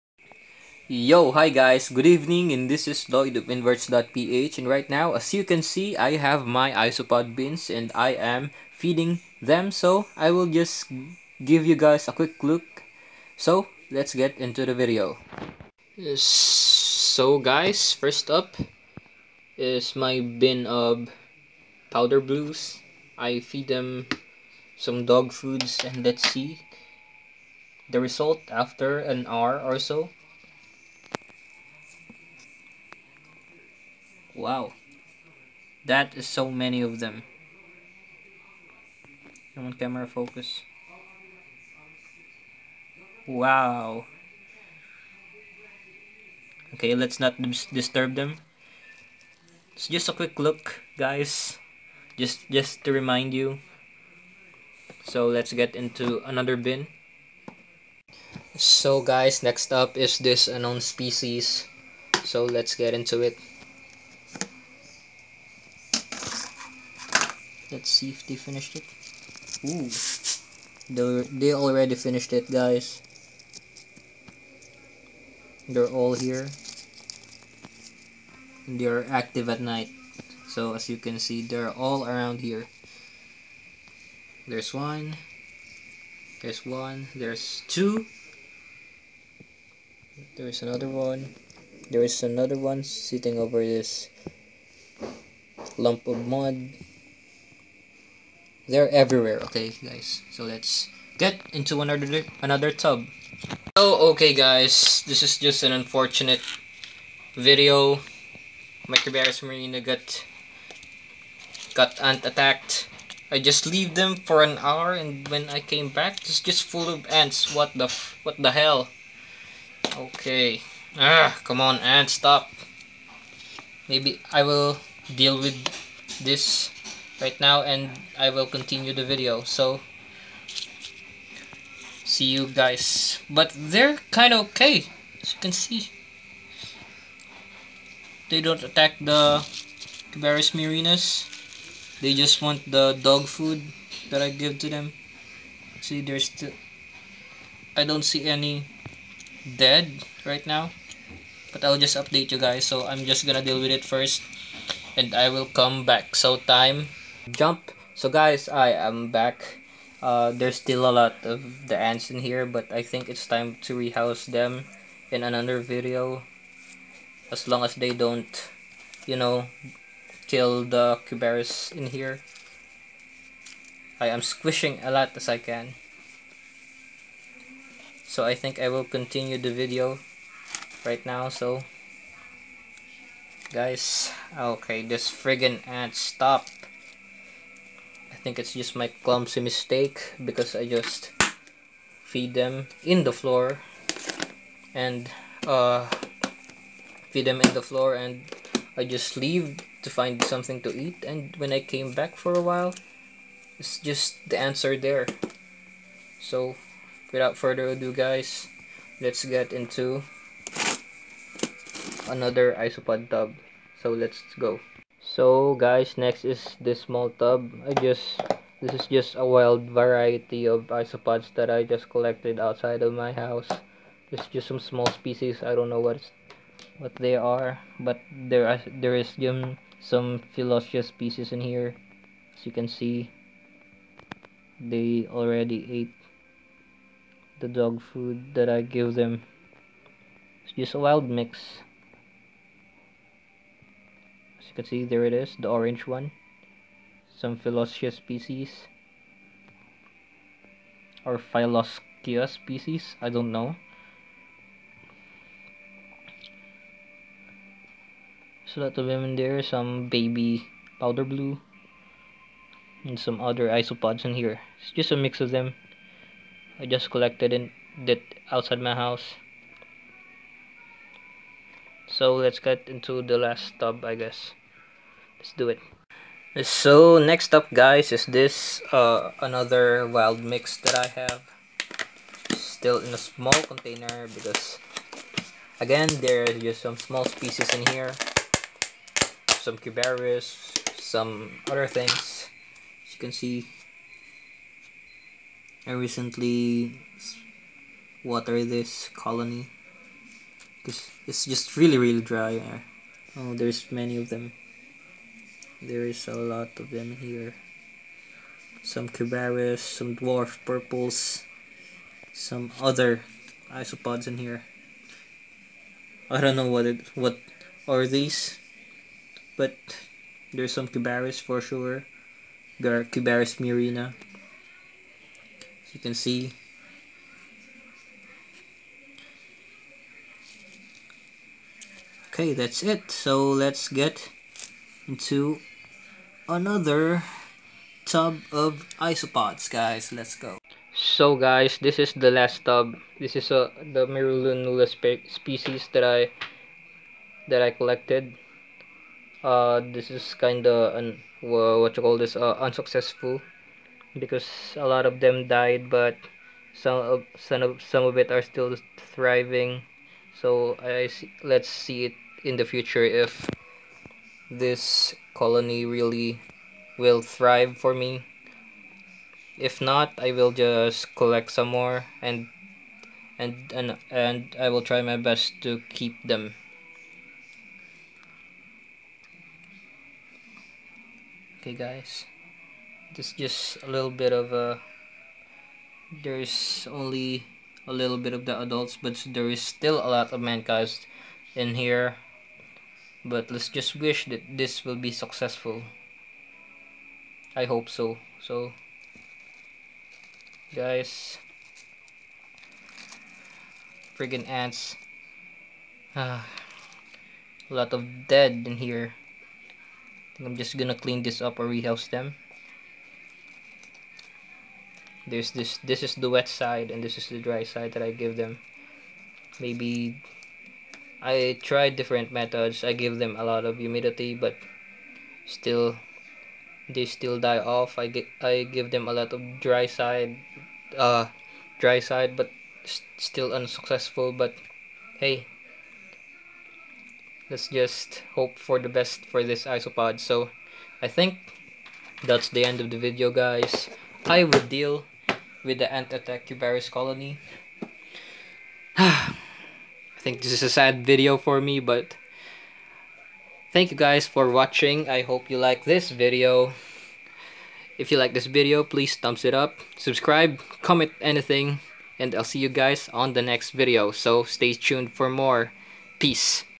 ANTS attacked my isopods tub! 😢
Hey guys just a quick update the isopods are all well now, they didnt attacked the isopods but the dog food only. Thank god i reacted quickly as soon as i see those damn ants haha

Isopods
Ant attack